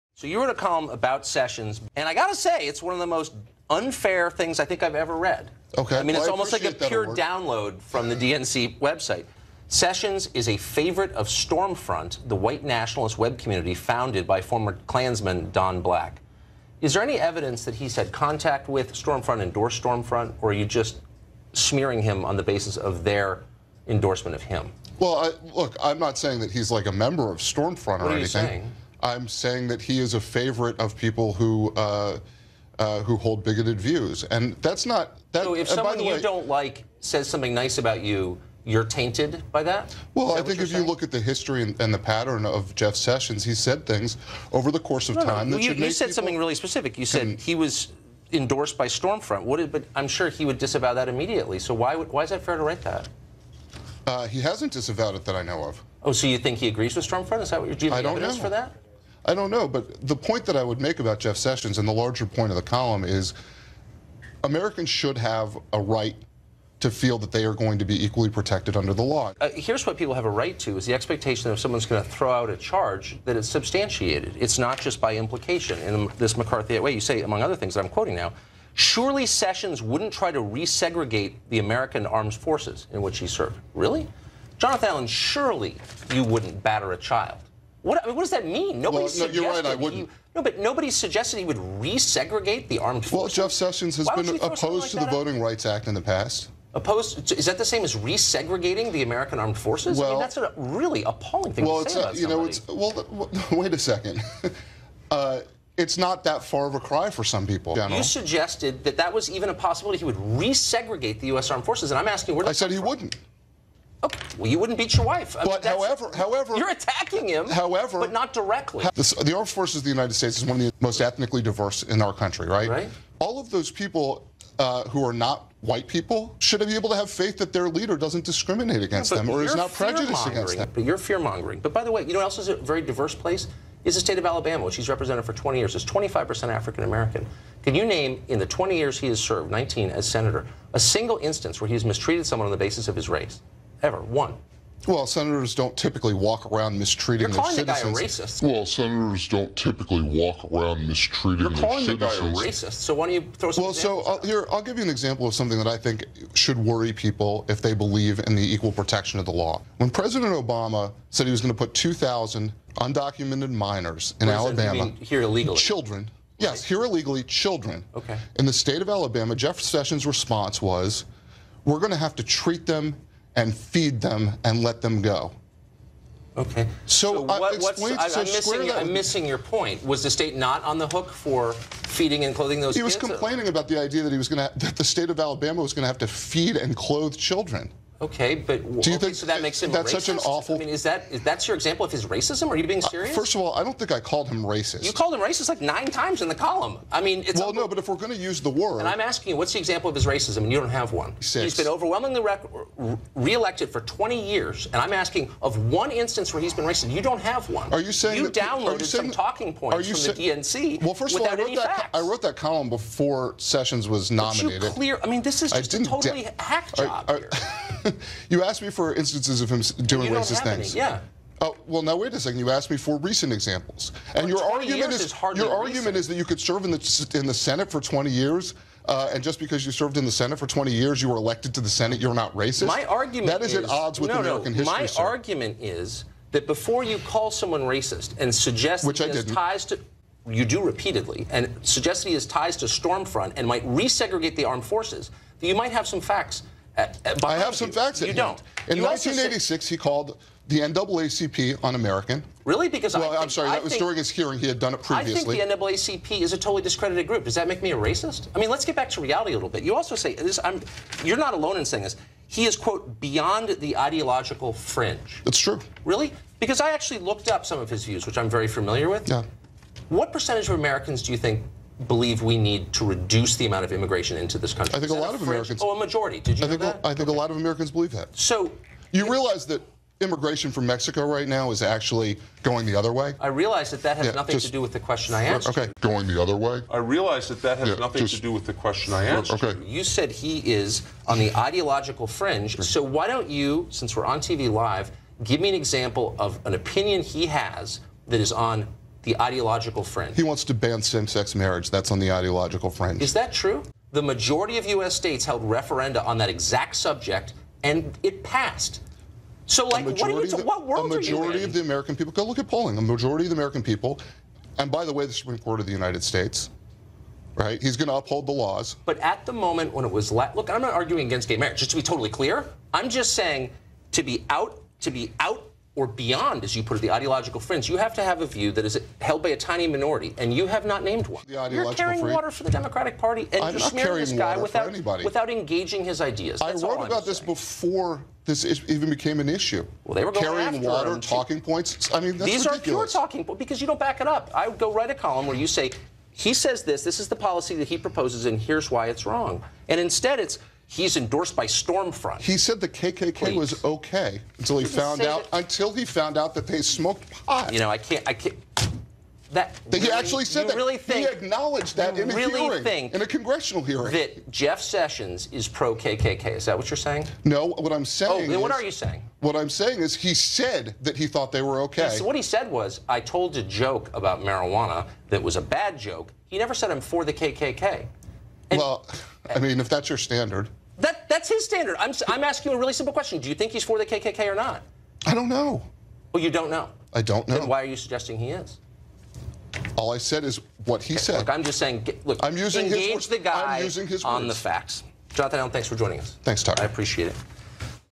Tucker Carlson RUINS journalist hack who claims Jeff Sessions is racist [Concise Version]
Tucker Carlson is a monster.  If he continues to bludgeon people like this, liberals might be scared to come on his show.